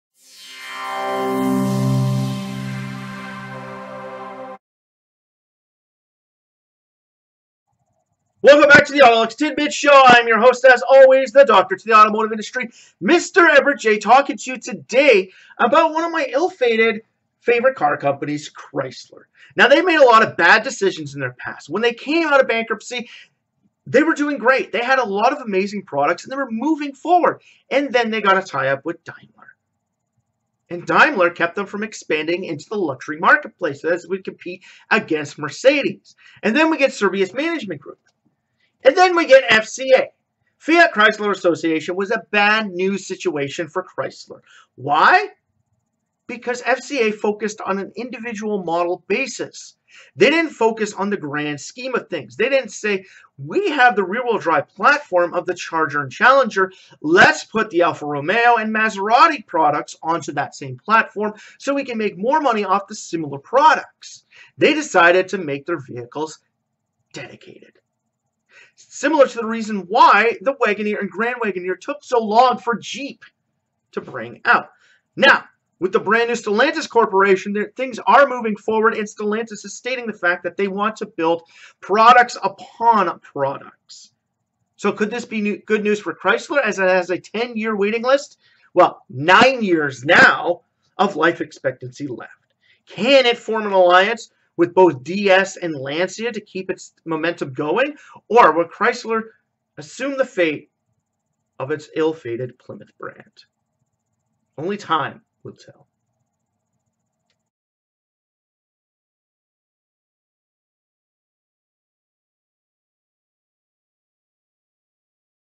Chrysler's Issues
Can anything help Chrysler out, or are they doomed to fade out at the end of the decade?  AutoLooks.net takes a look at their issues, to see if we can find a way to save them.